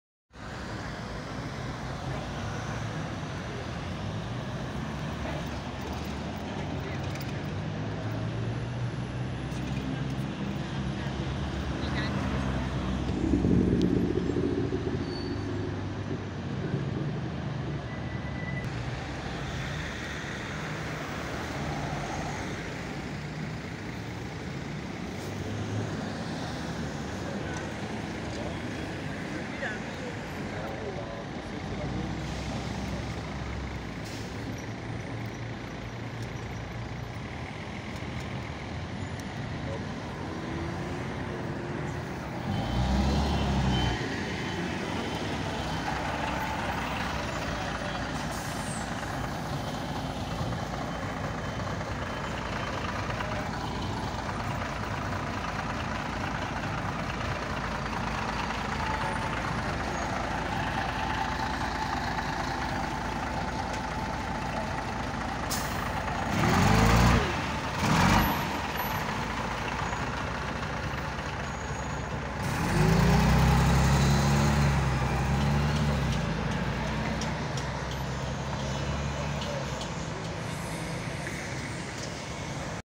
IKARUS 435 BPO-582 BKV BKK 7E VONALÁRA. BLAHA JÖN ÉS MEGY TOVÁBB A ÚJ NYÍR PALOTA FELÉ
Üdv a vezérnek jó egészséget kívánok baleset mentes napot kívánok önnek
VISZONYLAT 7E BUSZ.
TELEHELY CINKOTA AUTÓBUSZGARÁZS
EGYKORI KELENFÖLDI  KOCSI VOLT MÉG JÓ PÁR ÉVE